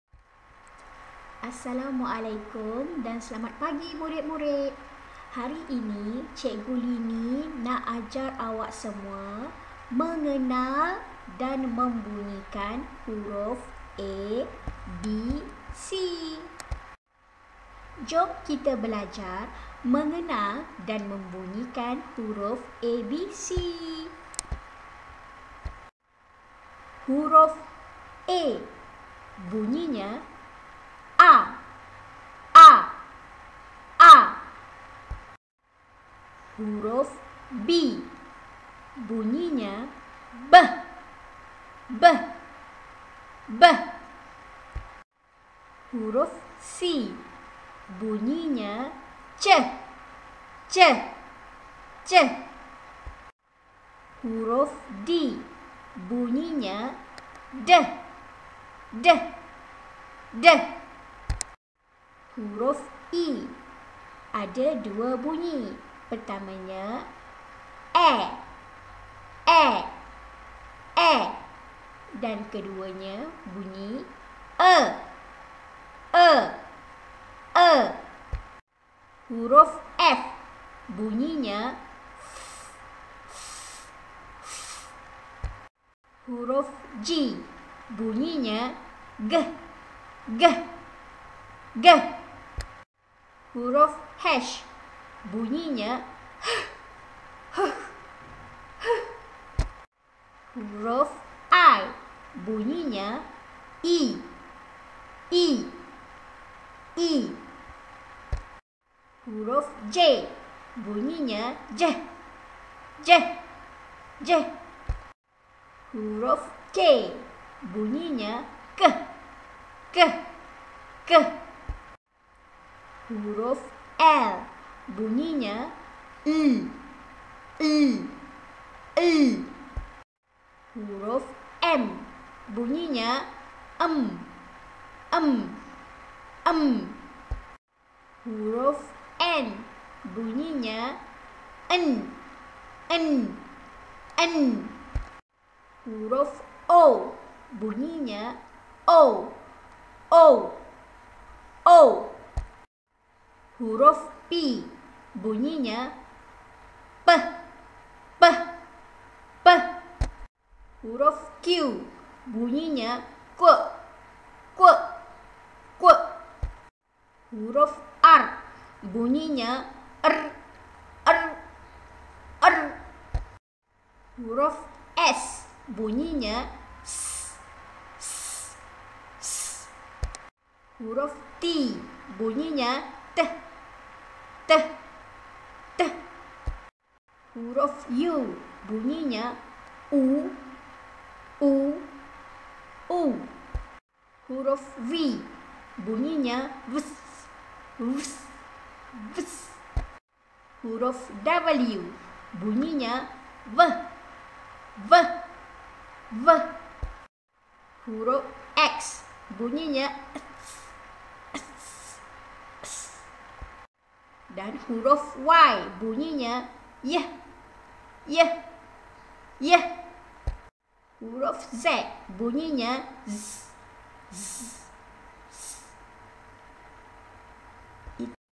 MENGENAL DAN MEMBUNYIKAN HURUF ABC
Pada akhir pengajaran, murid dapat :
1. Mengenal huruf A hingga huruf Z
2. Membunyikan huruf A hingga huruf Z dengan betul